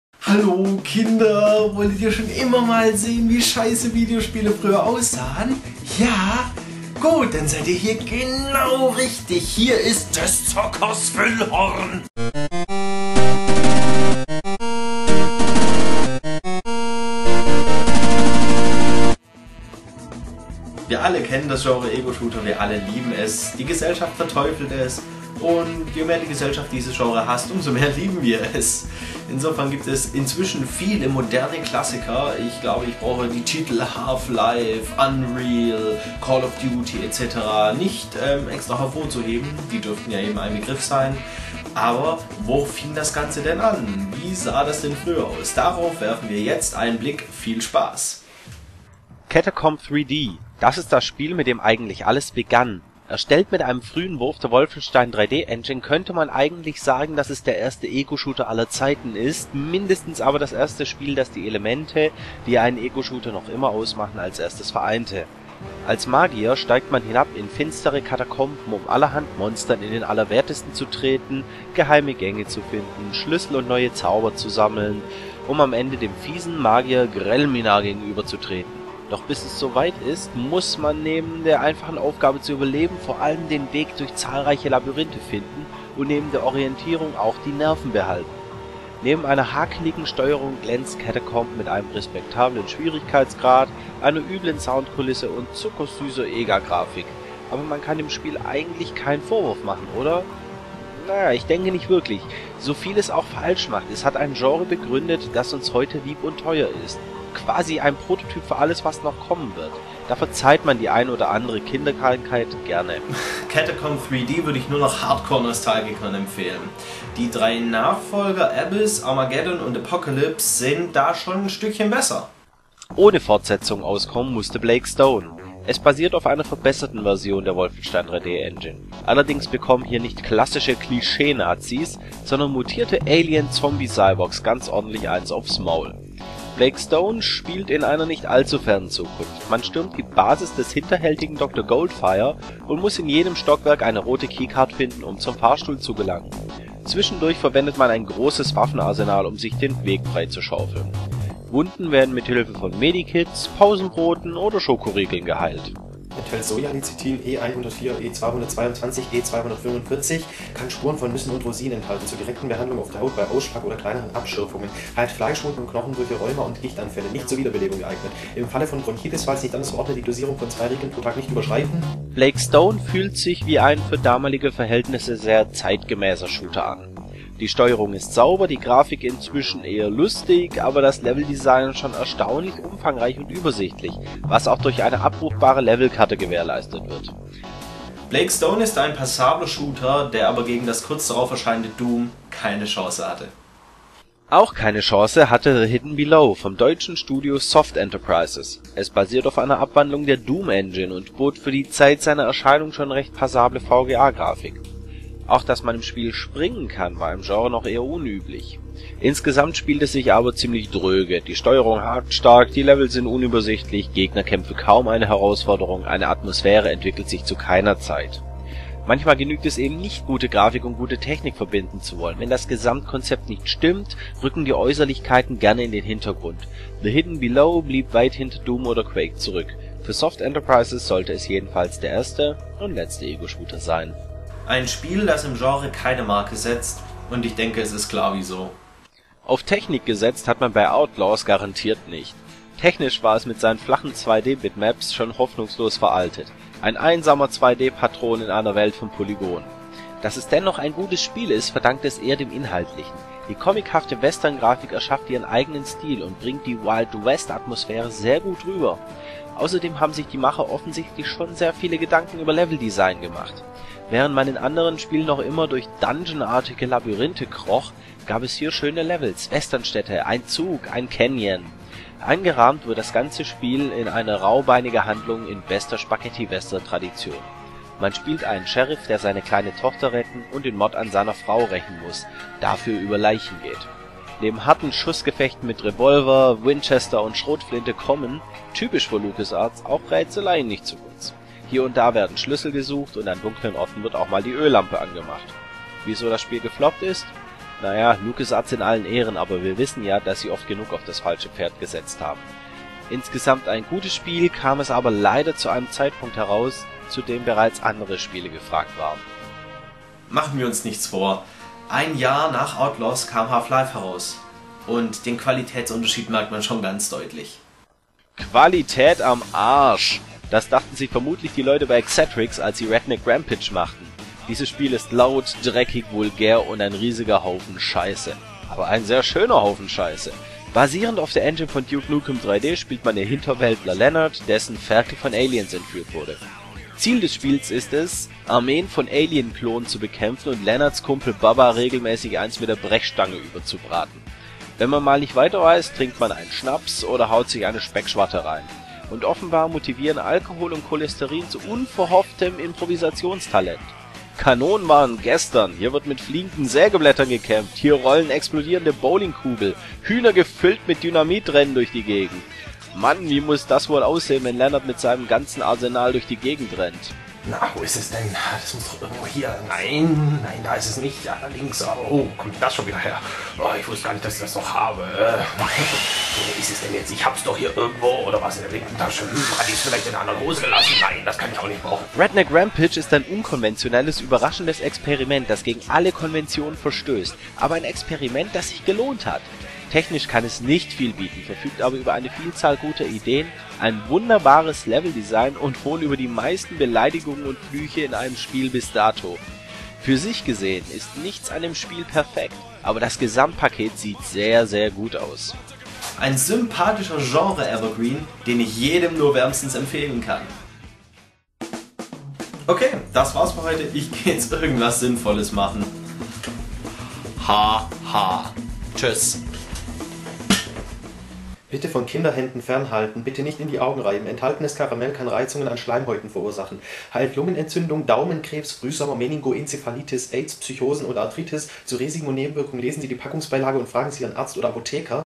Des Zockers Füllhorn - Retro Ego-Shooter
Nein, wir sind nicht die erste Generation von Zockern, die durch Ego-Shooter zu kriminellen Handlungen verführt werden. Aber damals war das irgendwie noch anders...